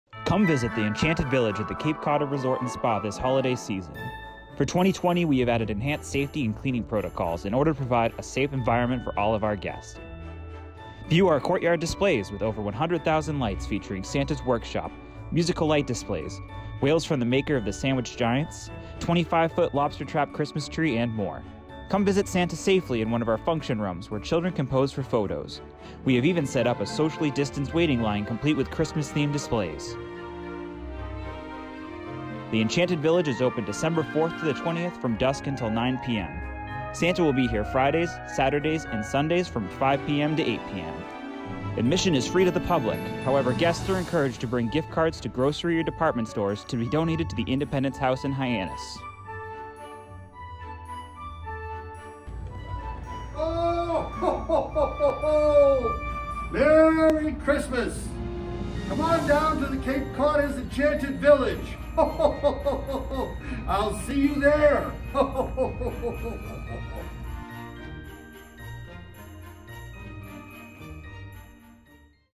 Cape Codder Enchanted Village 2020 - Amazing Lights, Decorations & Santa
Visit The Cape Codder Enchanted Village this year!  Many new displays have been added with Covid-19 safety protocols in place. Santa is here on weekends for socially distant visits.